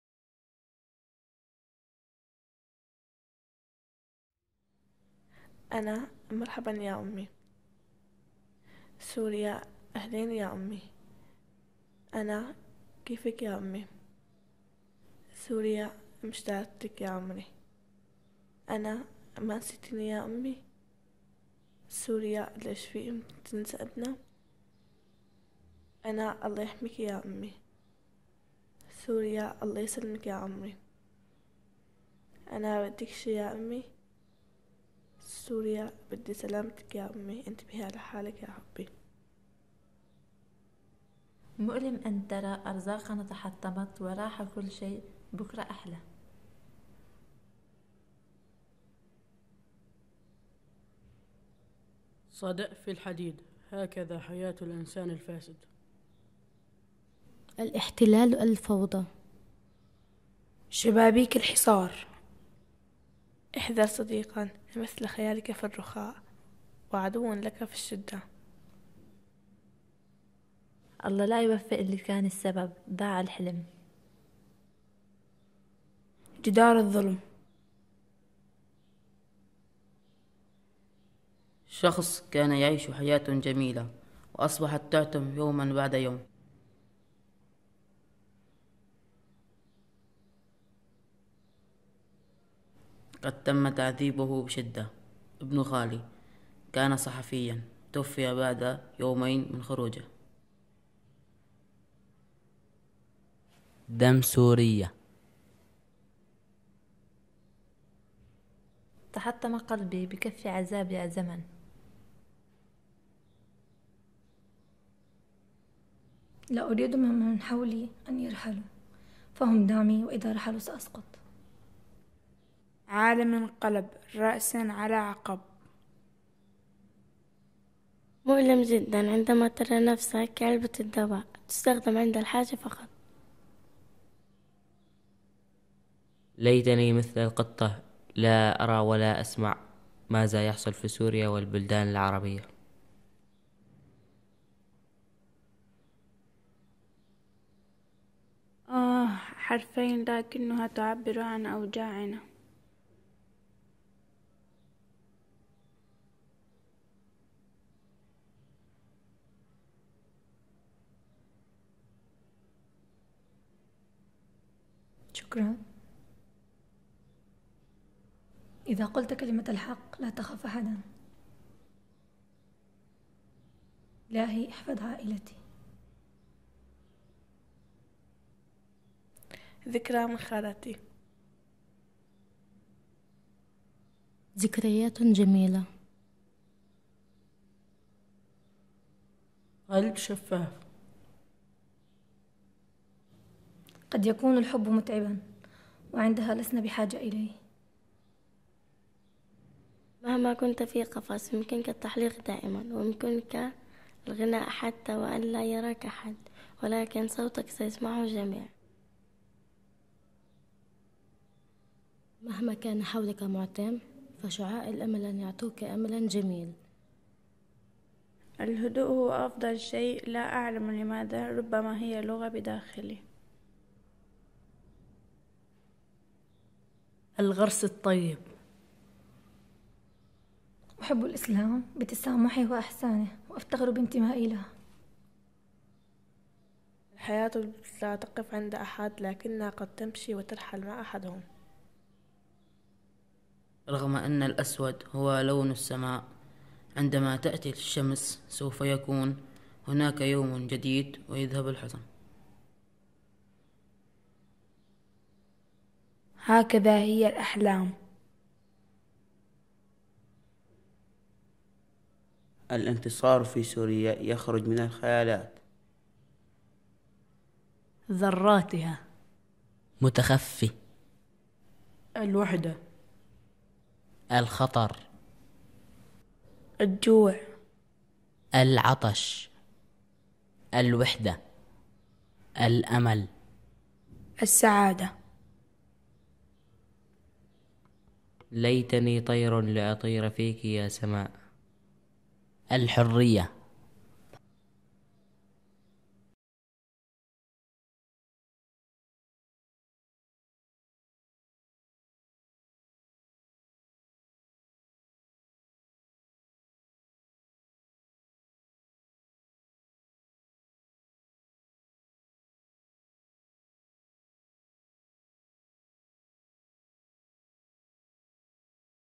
And We Shall Find Tales In The Shadows
World Vision partnered with PhotoVoice in May 2014 to run a participatory photography project with a group of Syrian refugee and host community children in Jordan.

The work produced is a real and personal reflection of the hopes of the young people, empowering their voices in the process.

The photographers are survivors of one of the biggest humanitarian crisis of this century. As with any crisis, children are always the most affected, losing their childhood through exposure to situations we cannot comprehend. Their lives have been transformed beyond recognition and any sense of familiarity or stability that should be associated with childhood has been snatched away from them. Through sharing their stories in this way, they allow us a unique glimpse into their lives.